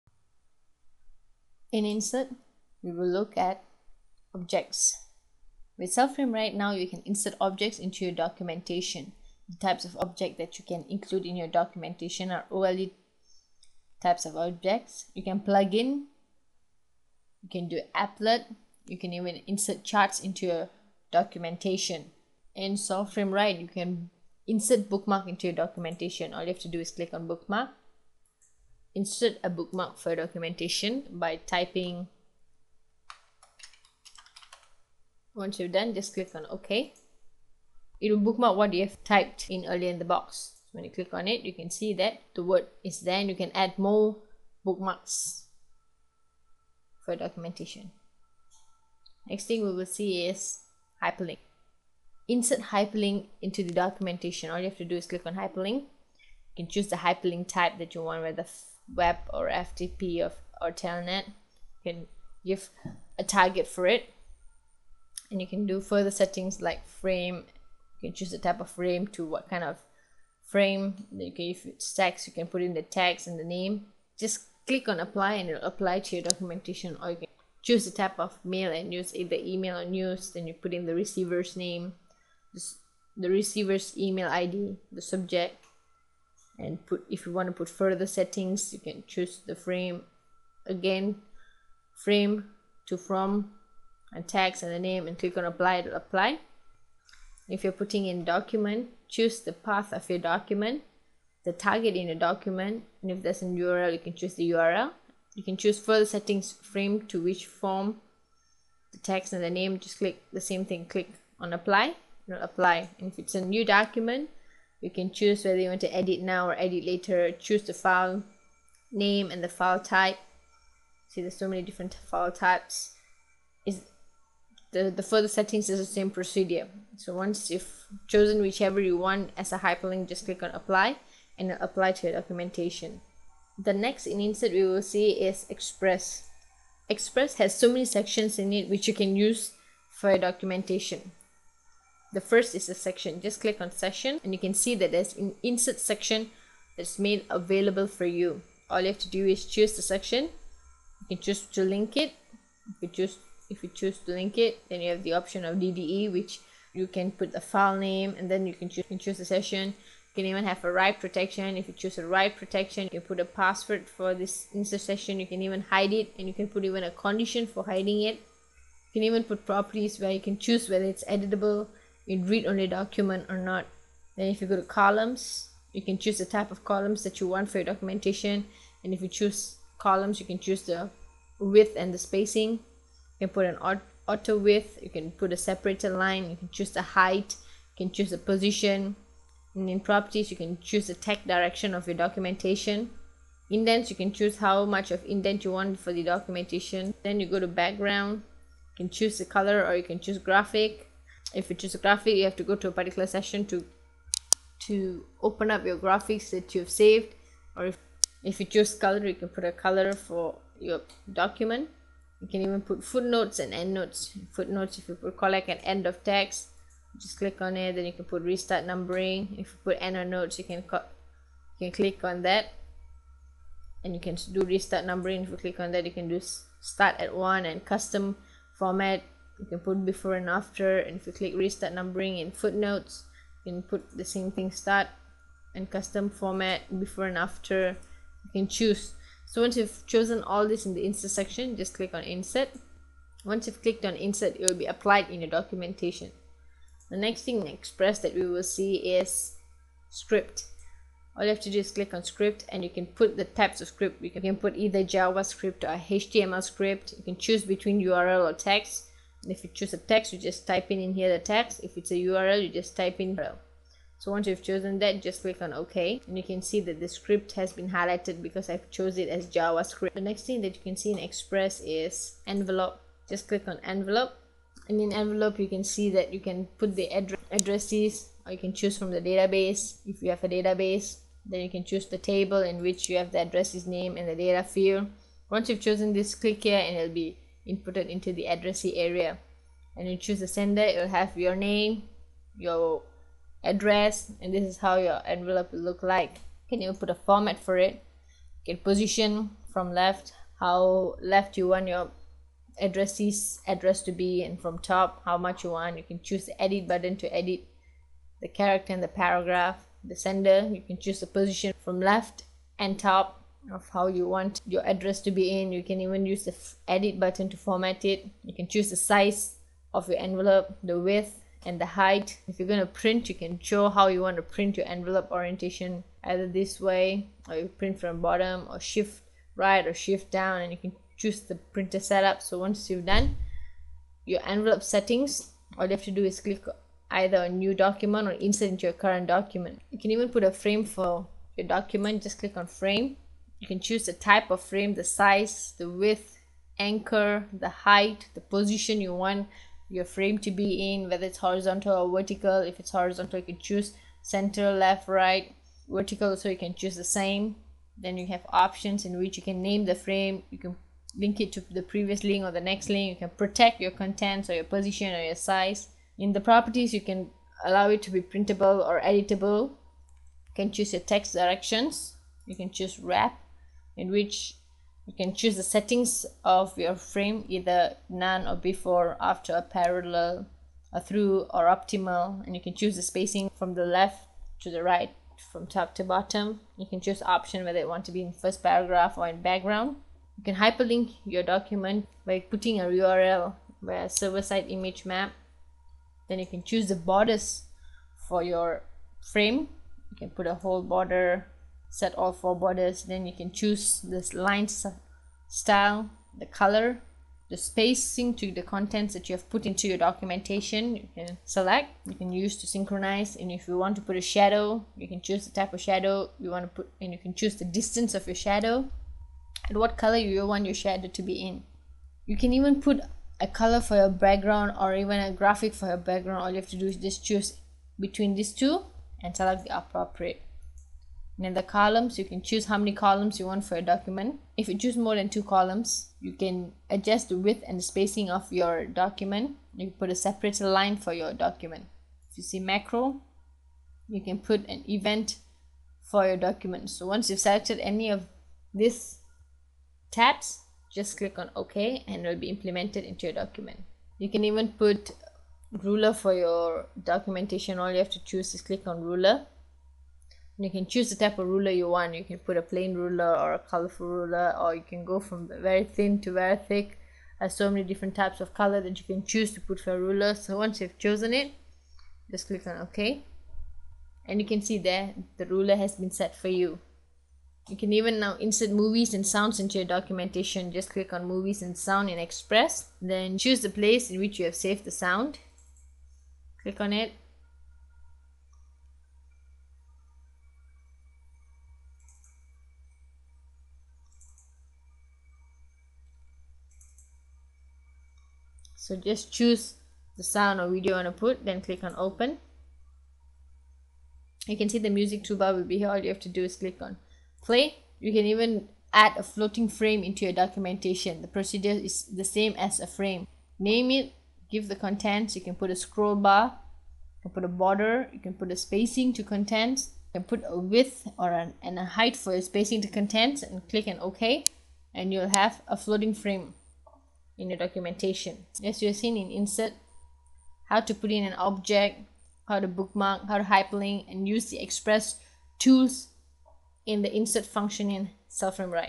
How to insert object, bookmark, hyperlink, and express?
This tutorial is made to help the users to insert object, bookmark, hyperlink and express into their Celframe Write document